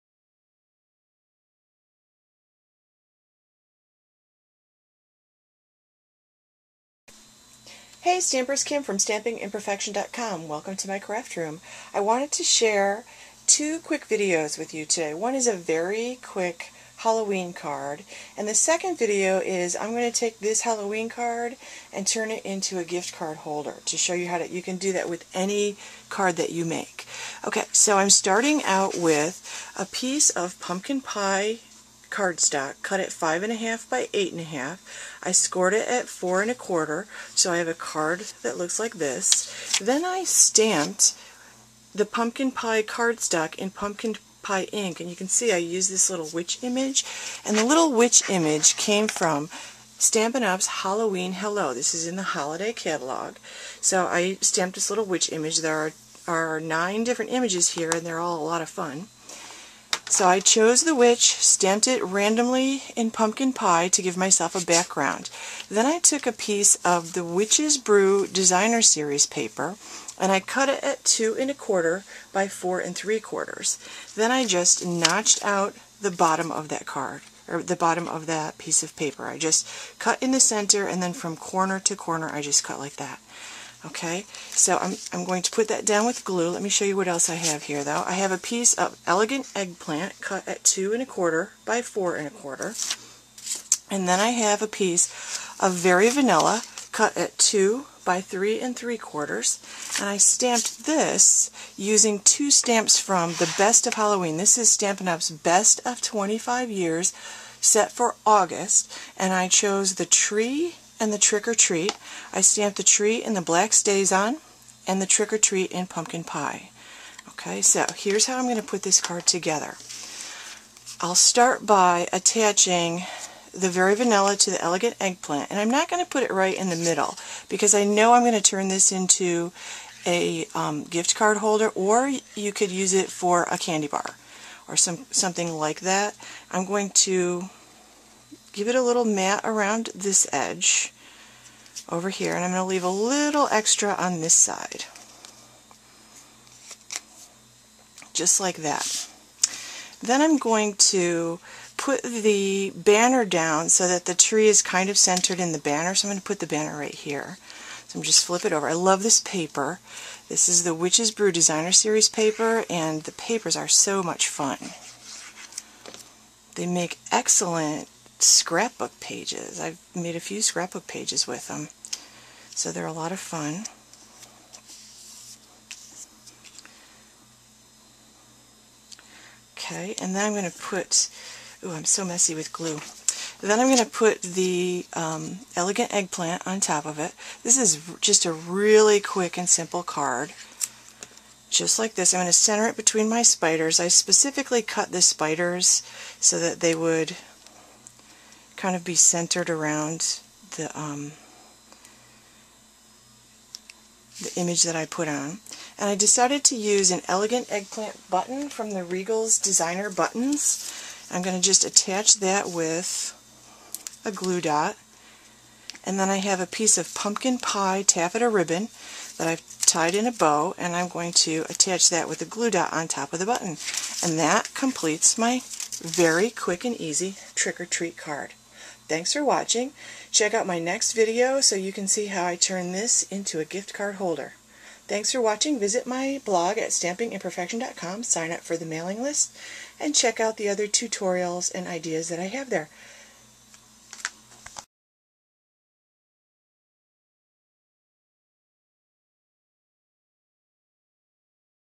Stampin' Up! Quick Card: Halloween
Make a very quick Halloween card using Halloween Hello and the Best of Halloween from Stampin' Up!